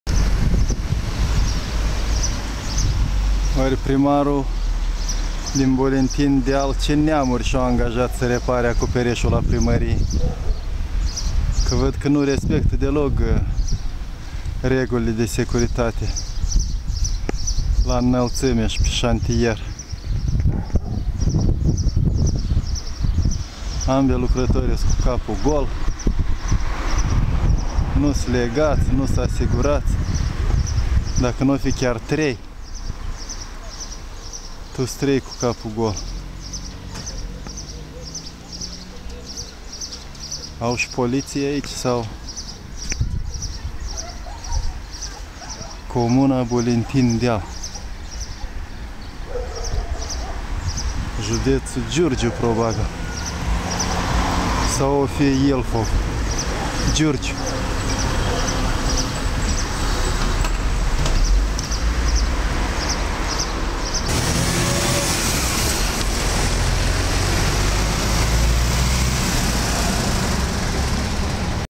Lucrează fără cască chiar pe acoperişul primăriei - Curaj.TV
Bolintin-Deal, judeţul Giurgiu, clădirea primăriei